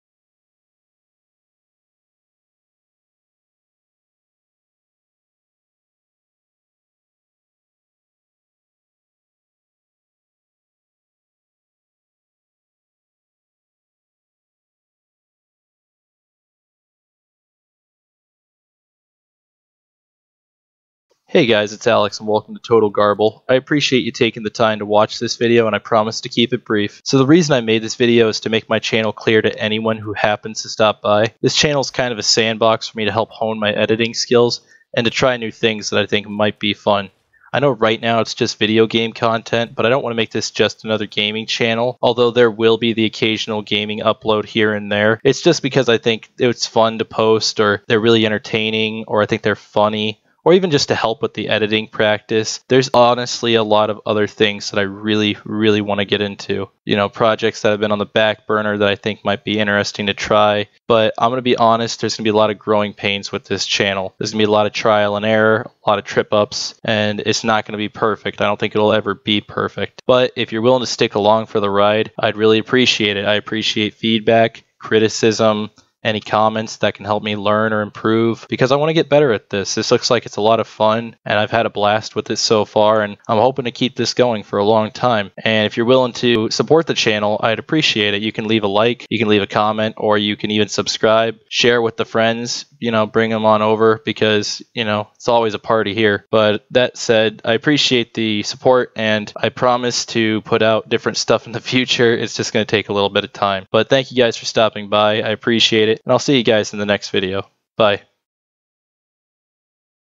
Mission Statement
Hey guys, it's Alex. This video pretty much describes what my channel is about and what I hope to do with it in the following months. I'm excited to be making videos and I hope to see more of you all in the future.

POST UPLOAD EDIT NOTE: I know I used the words "appreciate" and "honest" too many times. I didn't realize this until after the video had been mostly completed and after trying to record about 26 times, I didn't have the energy to do it again LOL. thanks for being understanding